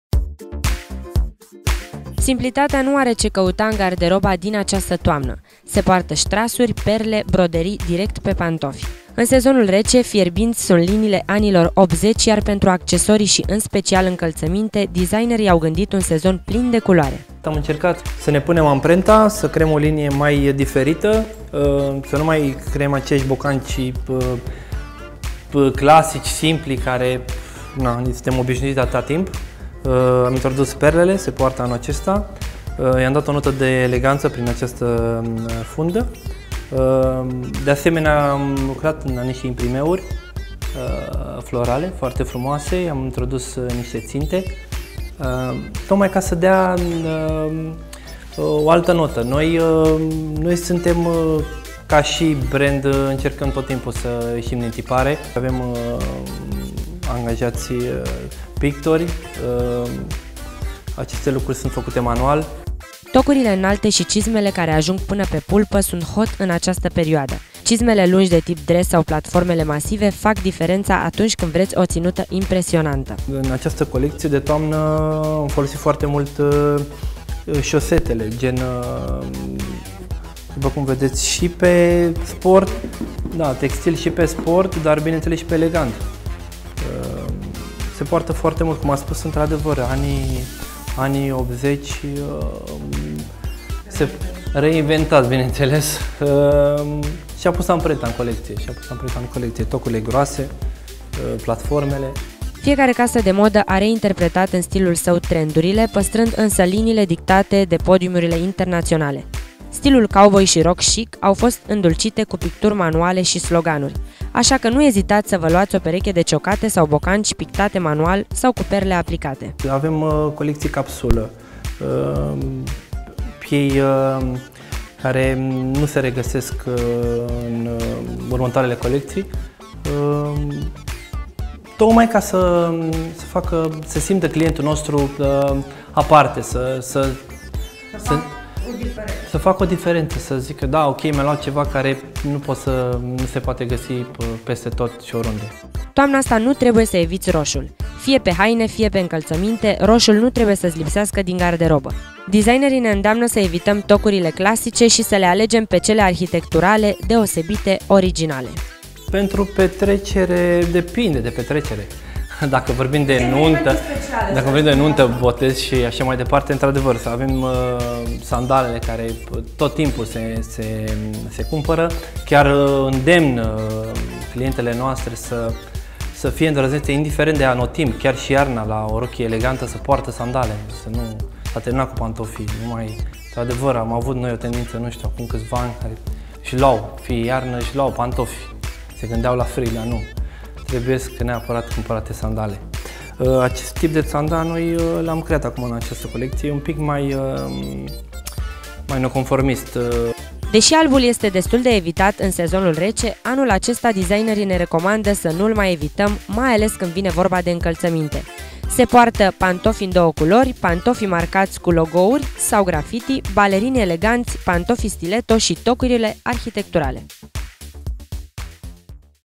Ionut Glavan, designerul casei de moda Mineli Boutique, pentru Secrete de Stil
Ionuț Glăvan, nume consacrat în industria modei pentru aportul pe care l-a adus în designul autohton de încălțăminte, are o relație specială cu shoppingul. Designerul privește acest fenomen în primul rând ca antreprenor, iar apoi sub forma unui client exigent, ce ar putea cu ușurință să îi invețe și pe alții cum să facă corect cumpărături.